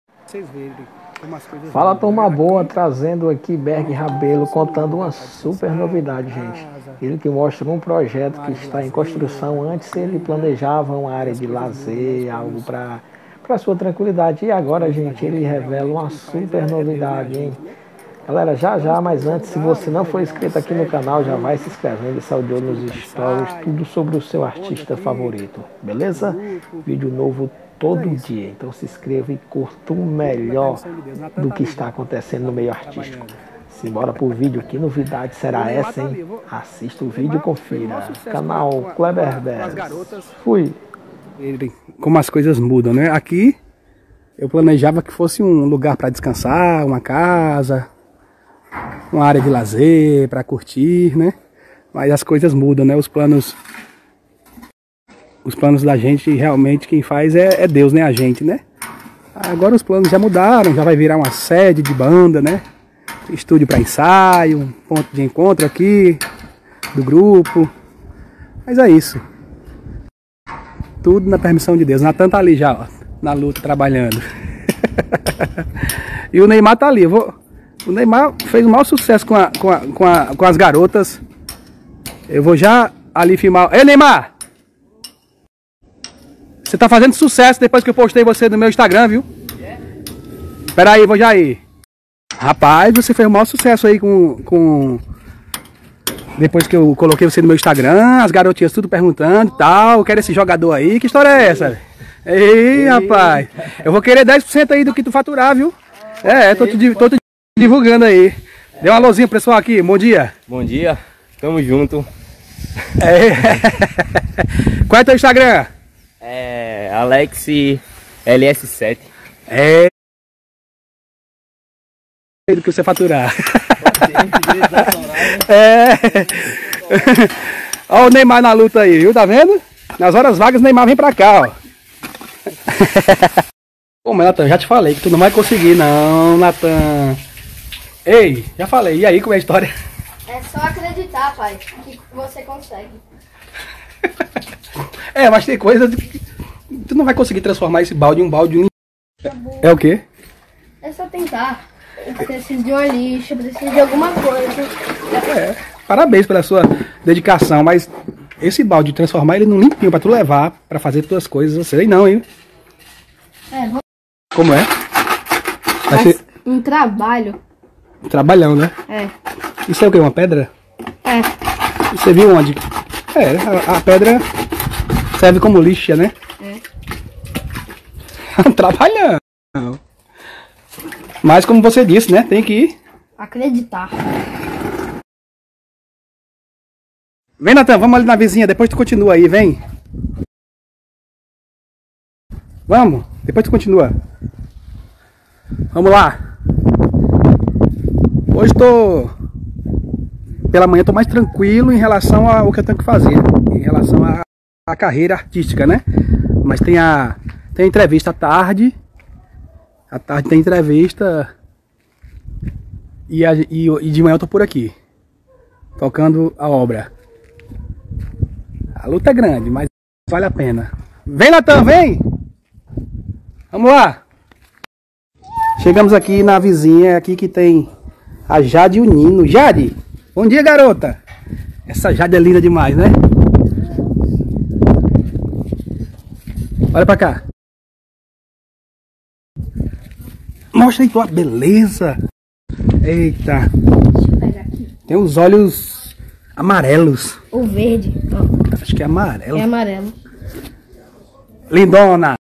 DE 👁️ NOS STORY/ hoje em seu Instagram BERGRABELO DEIXOU os fã muito felizes ao revelar ALGO 🙄😁
Veja a super novidade que BERGRABELO acabou de revelar